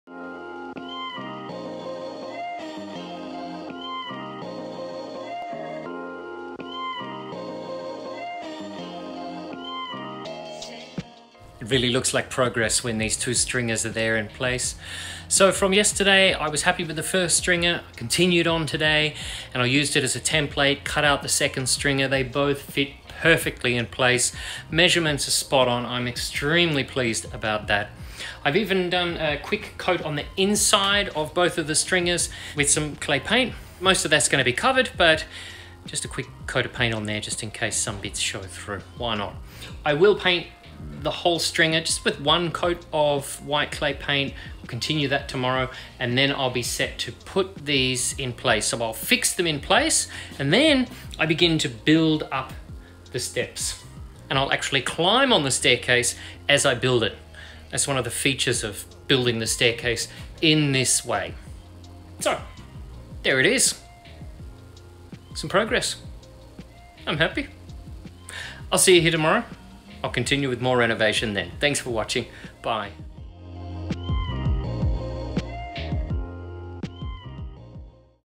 Renovation: attic staircase. Stringers cut and they fit!
This stage felt like the hardest. I hope that is the case and that this staircase is faster and easier to construct than the last one. Ask me in a week.